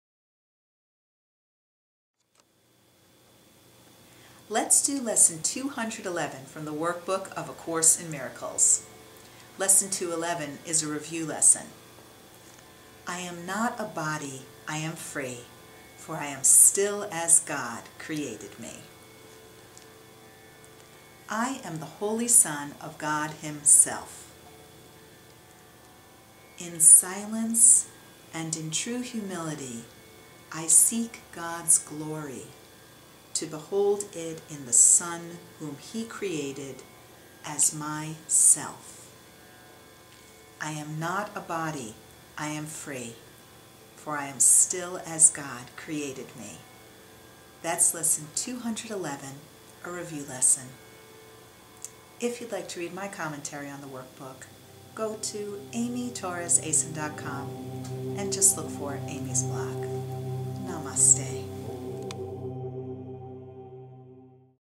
Workbook Lesson 211 from A Course in Miracles with Amy Torres
Lesson 211: I am not a body. I am free. For I am still as God created me.
Lesson 211 reviews Lesson 191: I am the holy Son of God Himself.